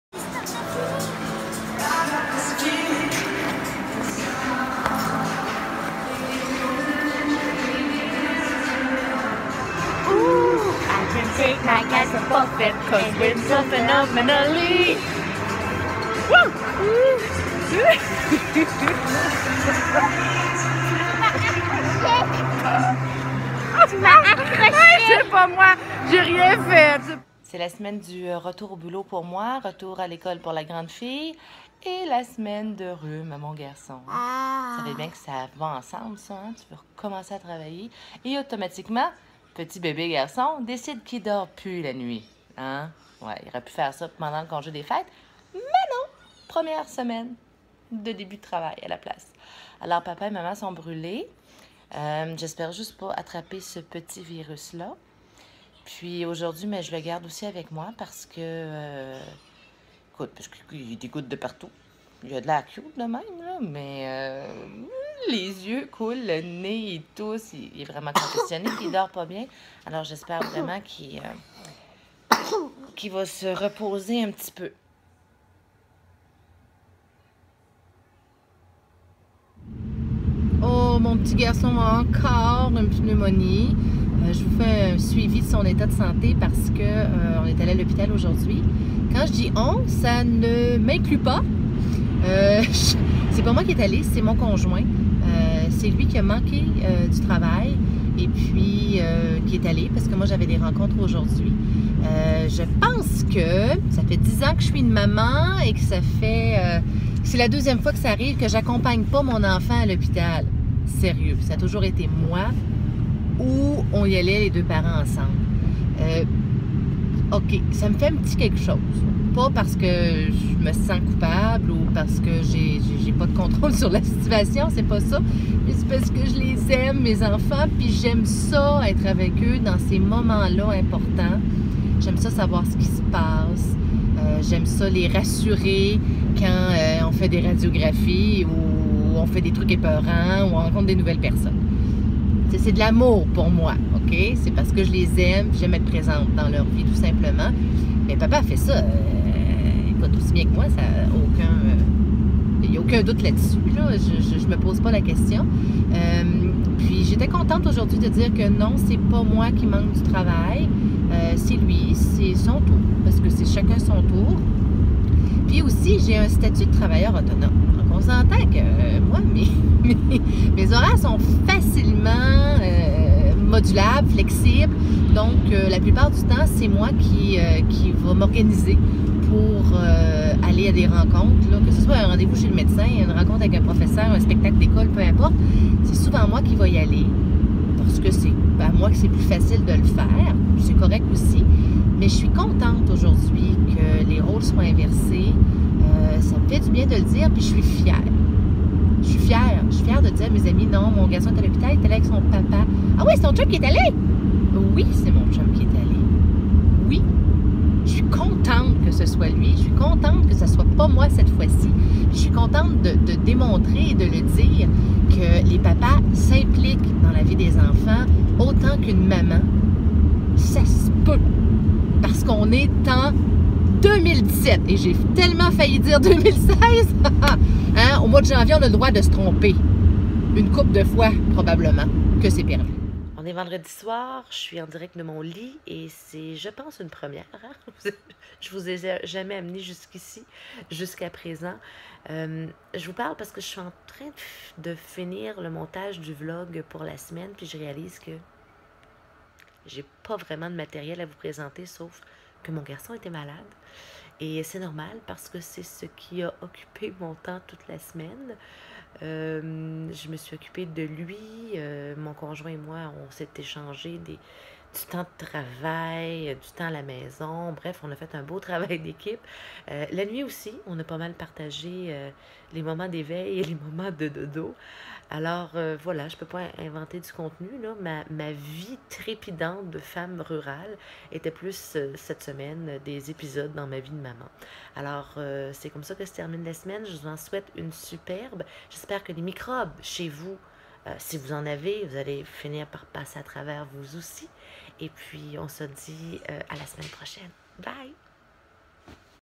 Patins et vilains microbes Vlog #12
Weekly vlog avec Cassiopée Dubois, disponible sur le blogue Néorurale.ca !

Semaine où je me suis essentiellement occupé de mon bébé malade. :(

Bonne semaine!